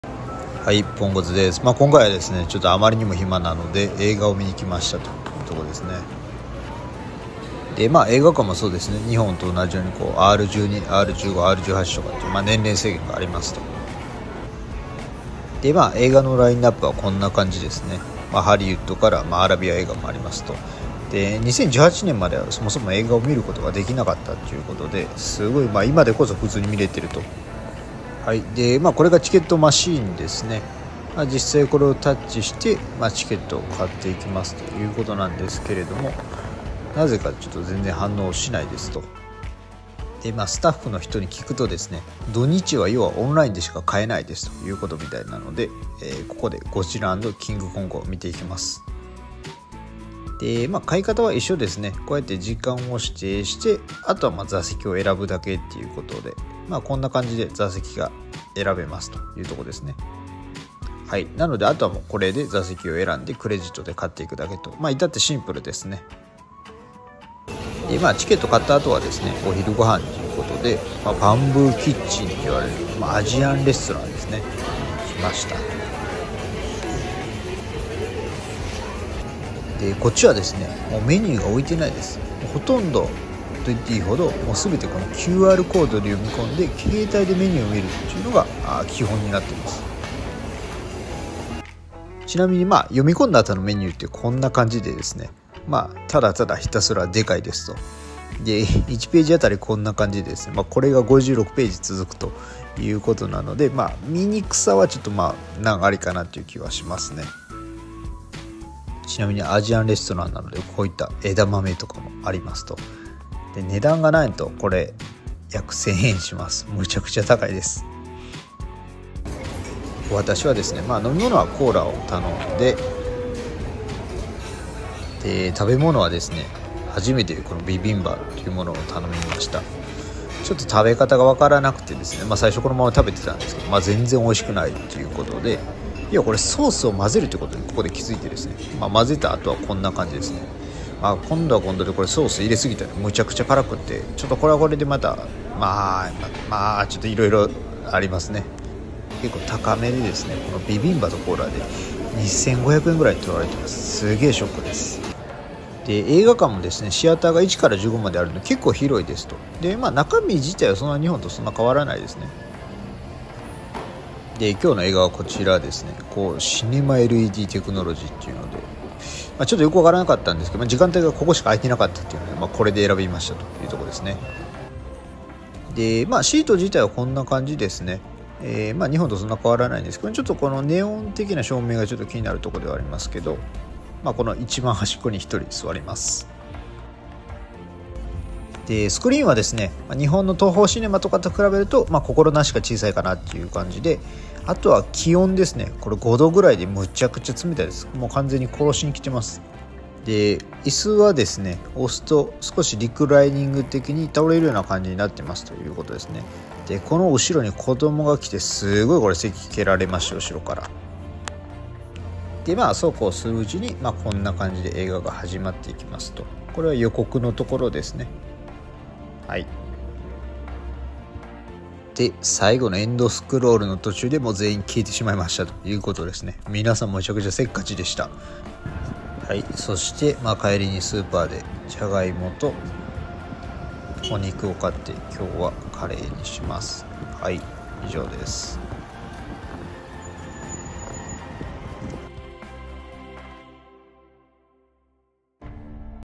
【ぼっち孤独】サウジアラビアでの映画館とは！30代 ルーティン
海外駐在(中東)で孤軍奮闘するぼっちのポンコツサラリーマンが、エリートサラリーマンを目指す動画です。非常に怠惰な生活を卒業し、アラビア語と英語の習得を目標とします！

今回はサウジアラビアでおっさんが１人 で映画デビューです。